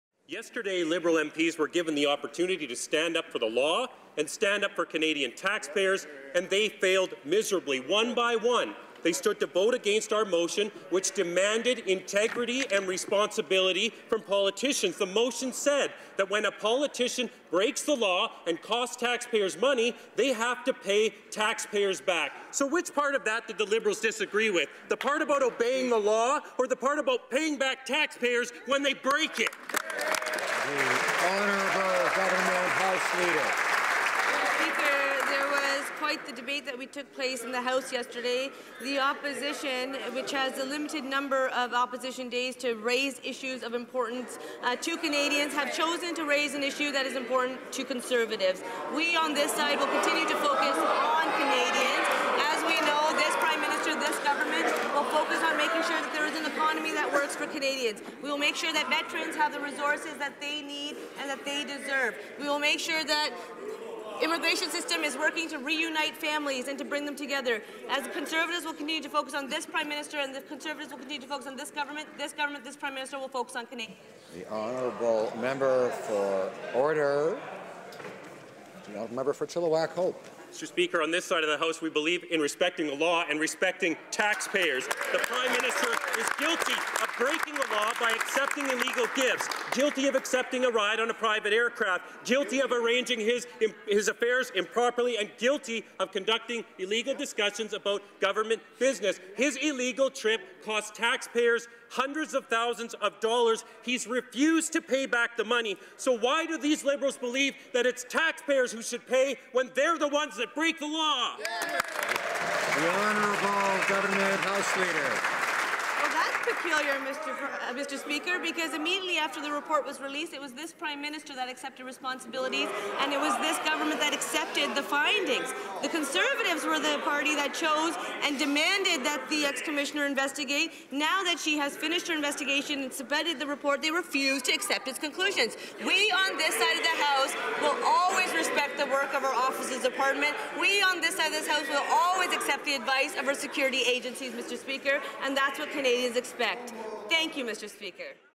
Pay it Back
Last night in the House of Commons every Liberal MP voted against this motion, which was brought forward by the Conservative Party:

"When any Member violates the Conflict of Interest Act, or violates the Conflict of Interest Code for Members of the House of Commons, and, in so doing, incurs a cost upon the taxpayer, that Member must repay those costs to the taxpayer."

Today I asked the Liberal MPs to explain why.